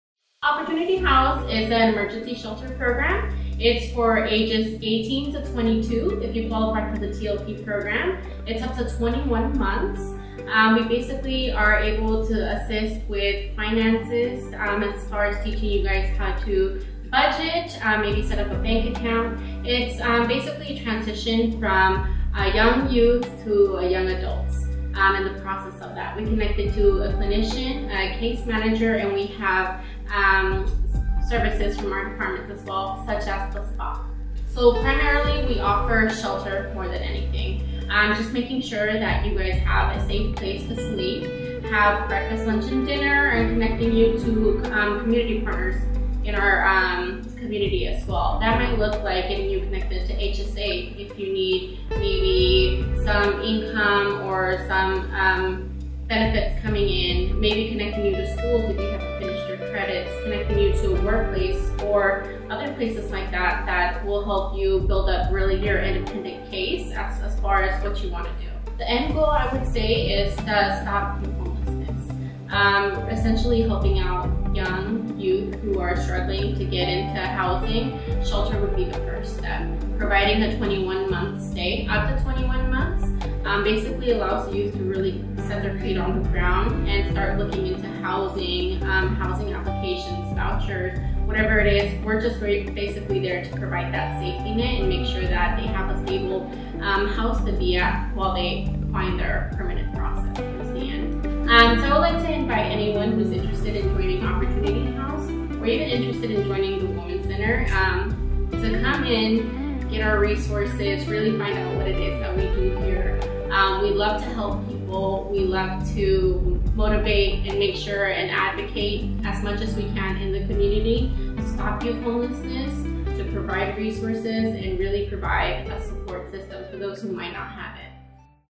Opportunity House Grand Re-Opening
Women's Center Youth and Family Services in Stockton recently held a grand reopening of their Opportunity House to benefit area youth between the ages of 18-22 in need of emergency housing. DMedia interns Anthony Villalobos and Ardon Aragon were in attendance to document the event.